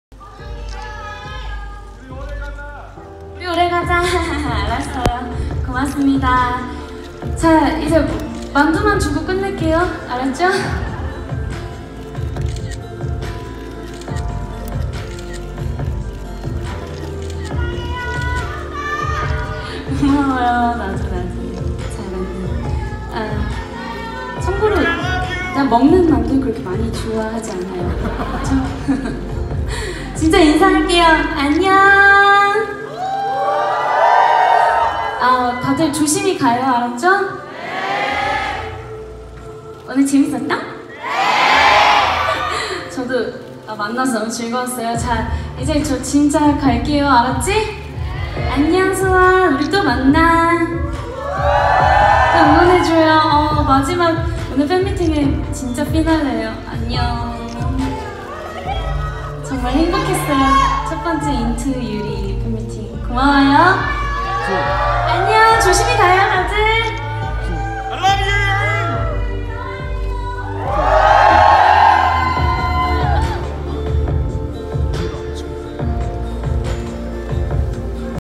2019.04.07 소녀시대 유리 첫 팬미팅 투어 INTO YURI 마무리멘트 동영상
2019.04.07 소녀시대 유리 첫 팬미팅 투어 INTO YURI 마무리멘트 동영상